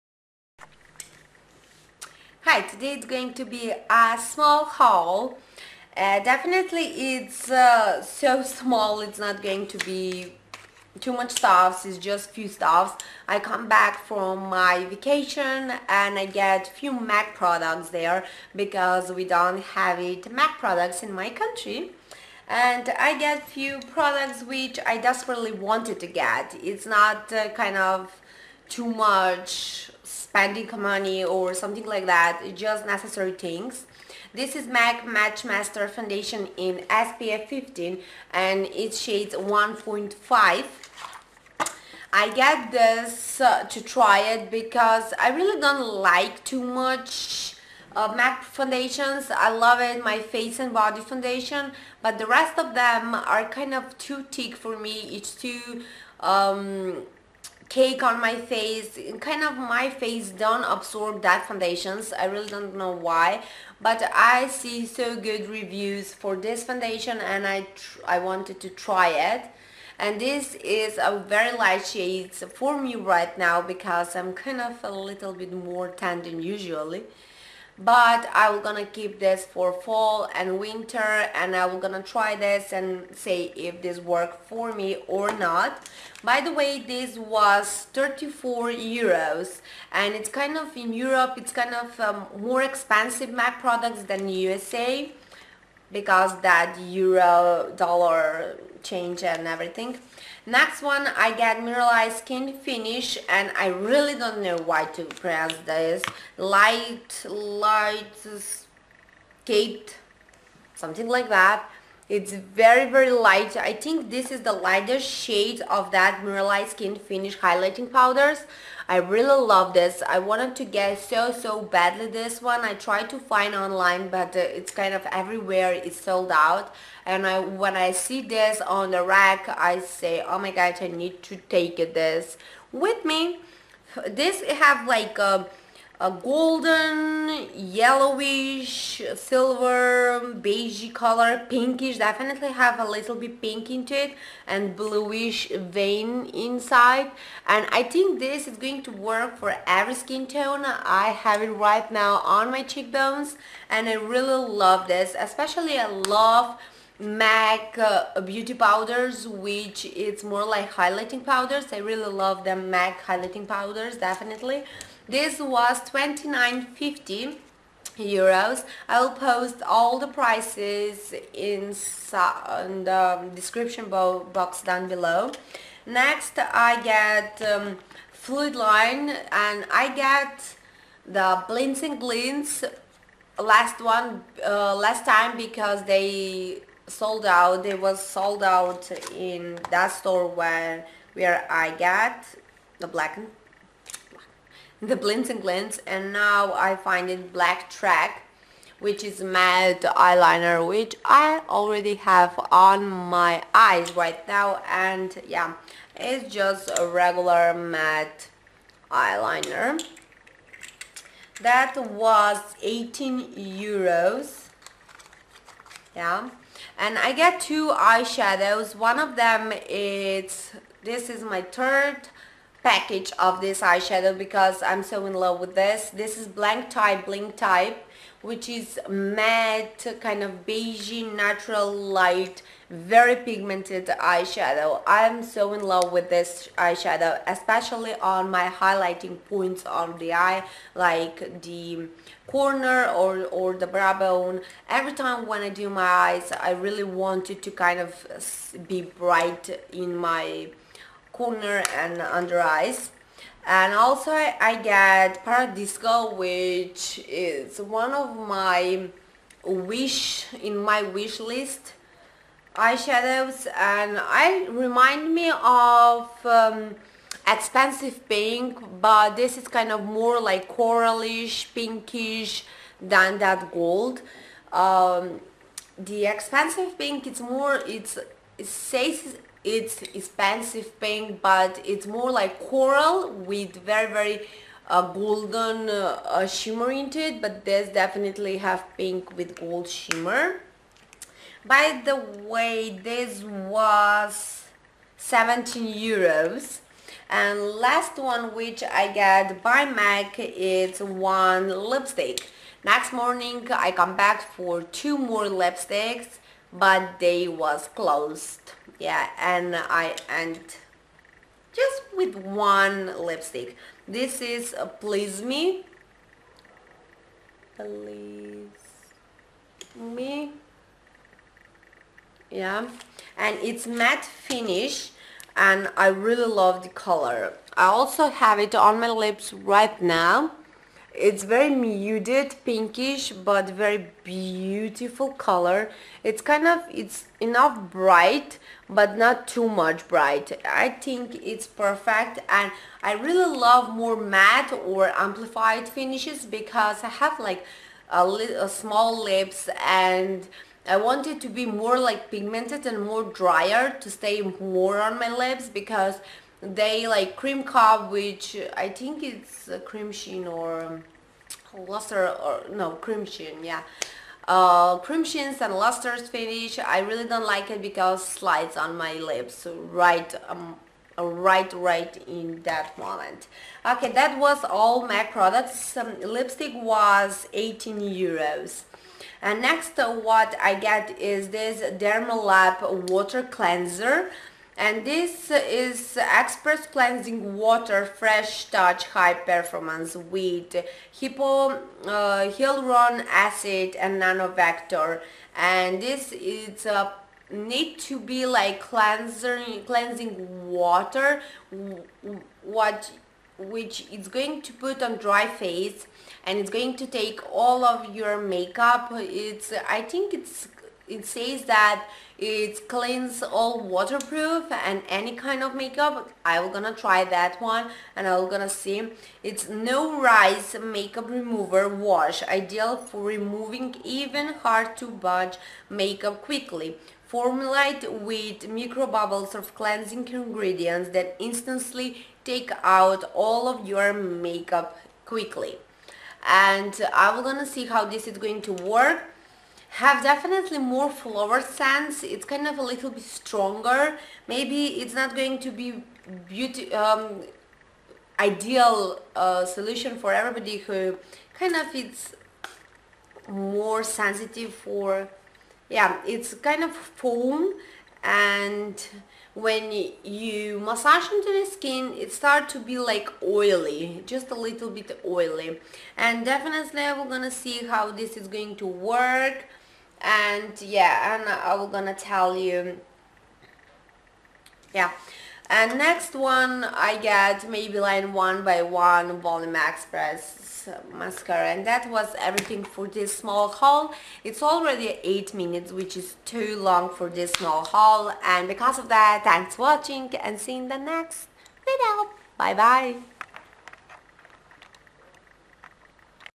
♥ Mini M.A.C. Haul ♥
mac matchmaster foundation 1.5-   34 euros
mac mineralize skinfinish lightscapade - 29.50 euros
mac fluidline blacktrack   -  18 euros
mac eyeshadow blanc type - 17 uros
mac eyeshadow paradisco - 17 euros
mac lipstick please me  - 18 euros